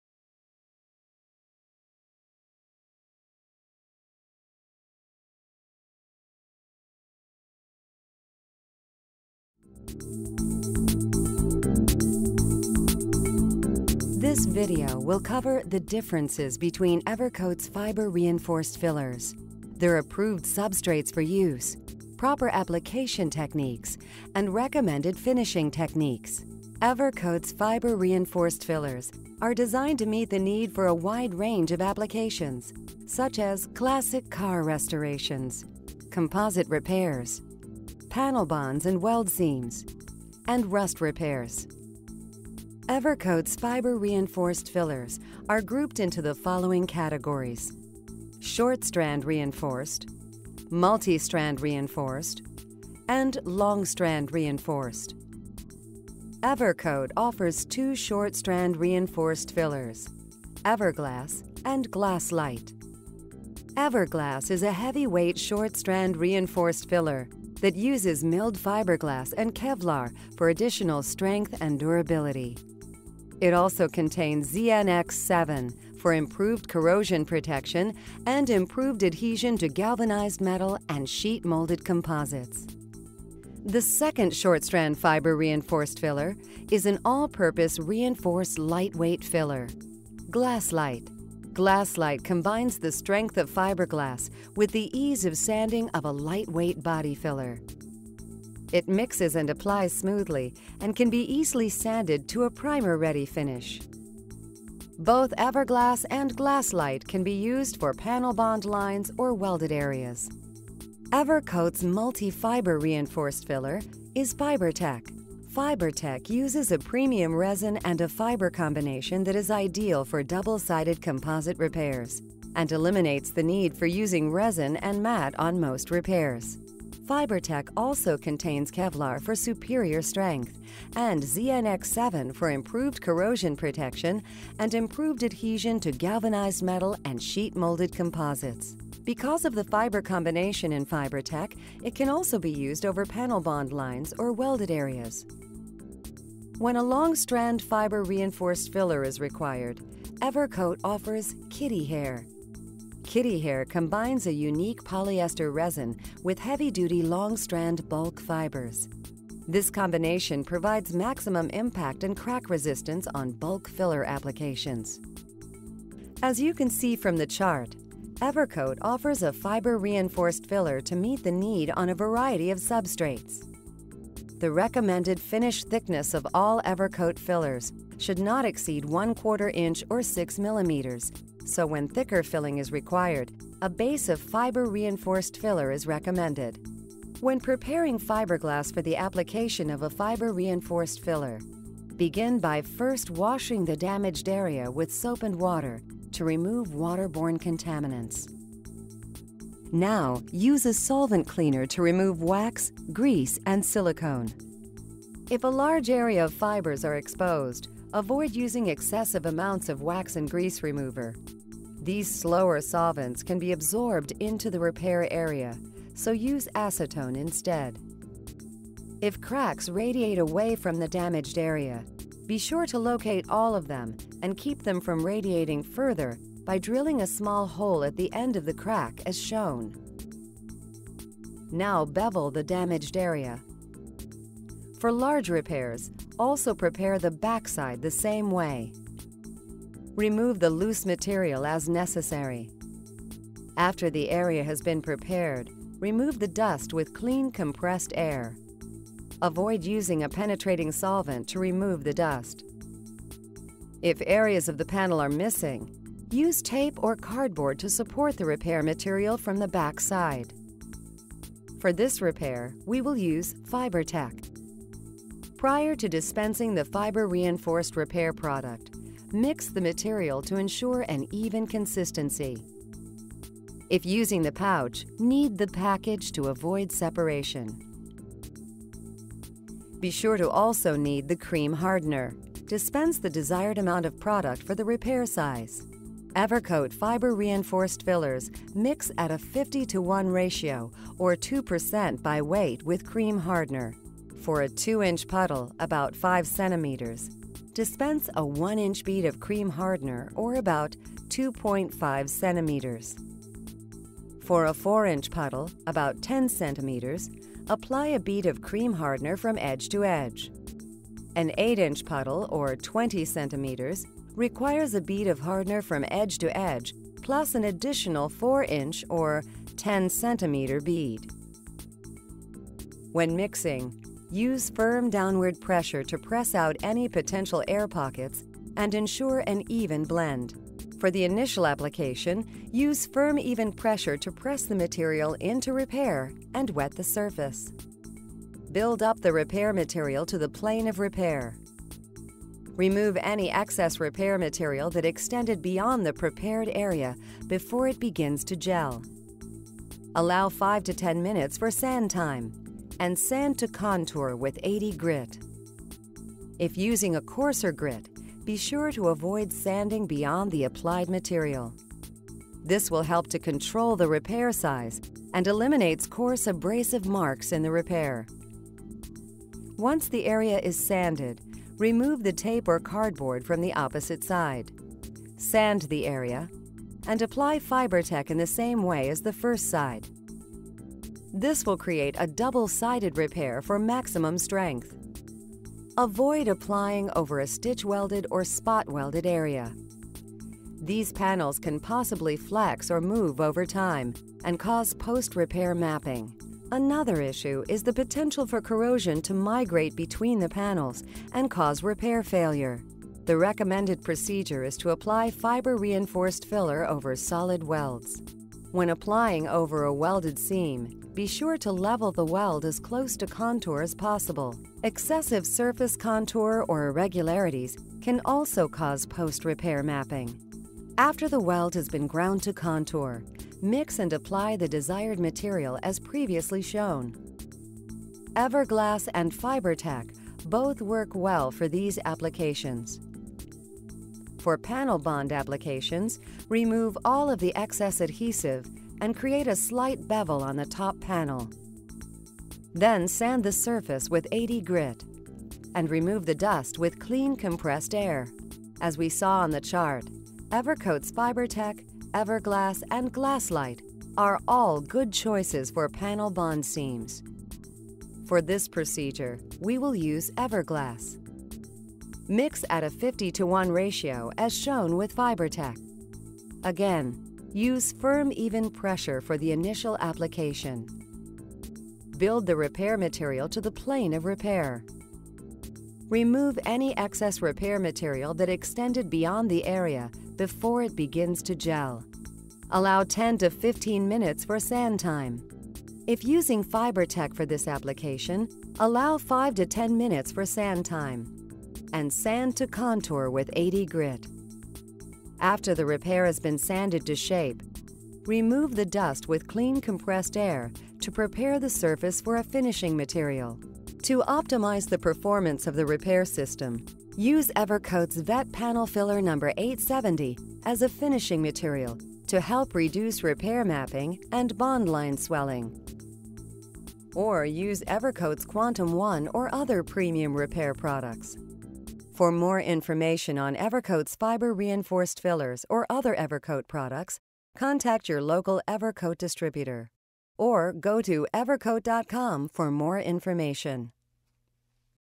EVERCOAT Fiber Reinforced Fillers
This video will cover the differences between Evercoat's Fiber Reinforced Fillers, their approved substrates for use, proper application techniques, and recommended finishing techniques. Evercoat's Fiber Reinforced Fillers are designed to meet the need for a wide range of applications such as classic car restorations, composite repairs, panel bonds and weld seams, and rust repairs.